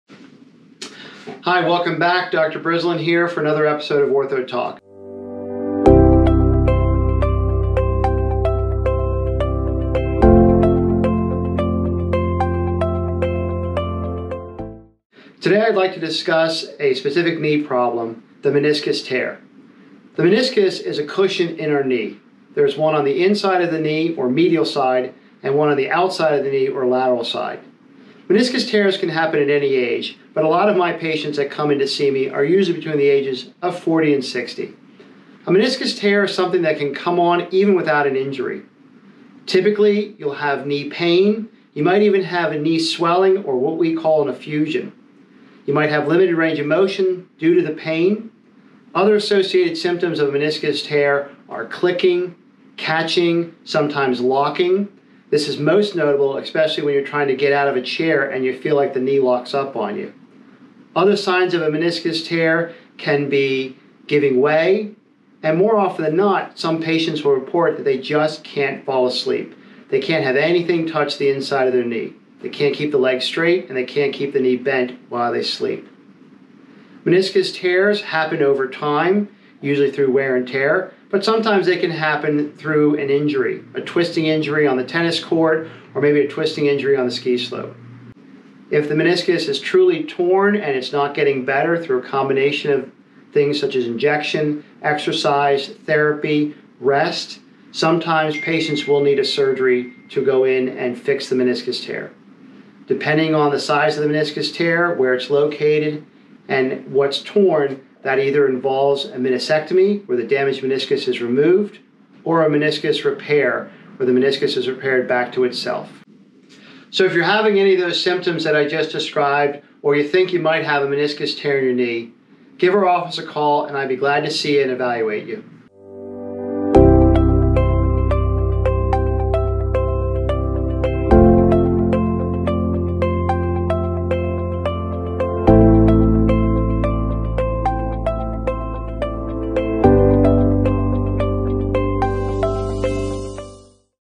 Ortho Talk Episode 2 - Meniscus Tears
In this episode of Ortho Talk Dr. Brislin goes over the common sings and symptoms of meniscus tears. Also discussed are different treatment options, including surgical intervention.